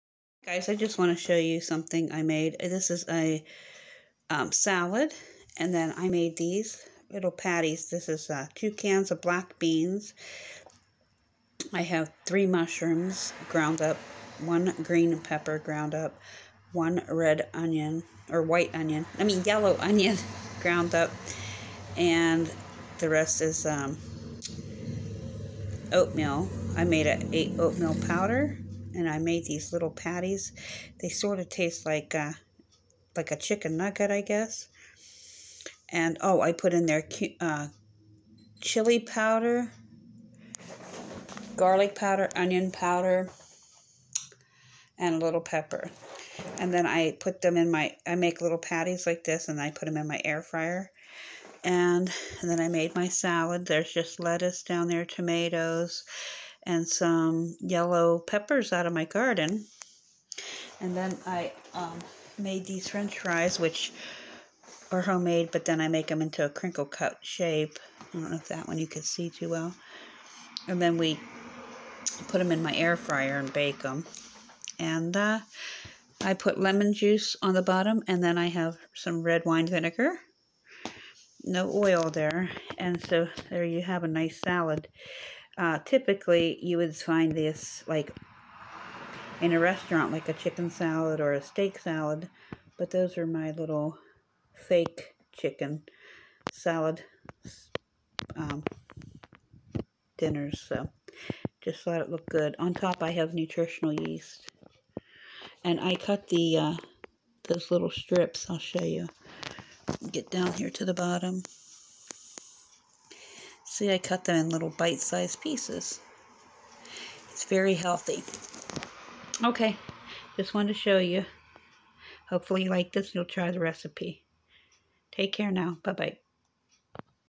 Air Fried Black Bean Nugget Salad With Air Fried French Fries - No Salt - No Oil
Air Fried Black Bean Nugget Salad with Air Fried French Fries

Recipe:

2 cans no salt black beans drained and blended
1-2 tsp chili powder
1 tsp black pepper
1 tsp garlic powder
1 tsp onion powder
3 whole fresh mushrooms blended
1 medium yellow onion blended
1 sweet yellow pepper blended
2 cloves of fresh garlic blended
2-3 cups of oat flour ( whole oats blended)
Splash Of Water to blend

1. Using high speed blender blend all ingredients except beans.

2. Use pastry cutter to smash beans.

3. Mix everything in bowl and make to the desired size.  I did round circles on shape of nuggets.

4. Using an air fryer on roast 400 degrees lay a piece of foil then some nuggets and roast for 8 minutes. Flip and roast another 5-8 minutes.

Will be hard.  Hint : Put them into a gallon size storage/ freezer  bag to soften while cooking the rest.

Don't worry,  it won't melt the bag, but it will regain moisture from the steam.

Serve whole, or cut into strips for salads, like I did 😉

Original recipe.